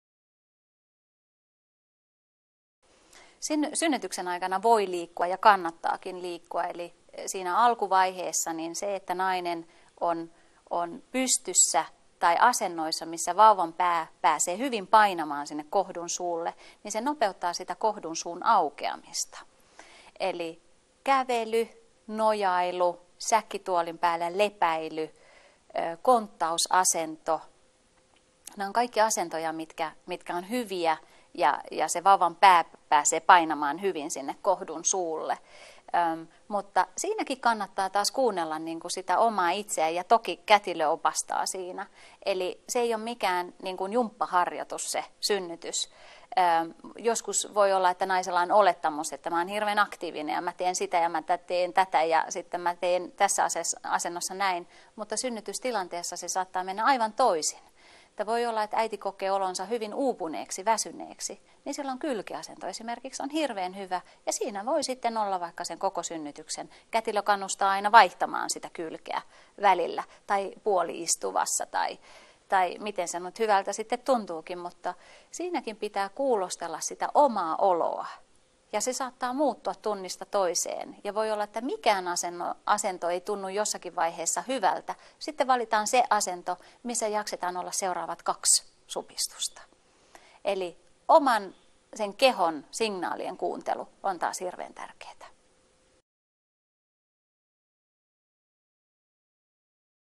Voiko synnytyksen aikana liikkua?
Synnytyksen aikana voi ja kannattaa liikkua. Alkuvaiheessa pystyasennossa oleminen nopeuttaa kohdunsuun aukeamista. Kävely, nojailu ja konttausasento ovat kaikki asentoja, jotka edistävät kohdunsuun aukeamista. On kuitenkin erityisen tärkeää kuunnella omaa itseään ja omaa kehoaan sekä kätilön ohjeita. Synnytys ei ole jumppaharjoitus ja on hyvä kuulostella omaa oloa, joka saattaa muuttua useita kertoja synnytyksen aikana.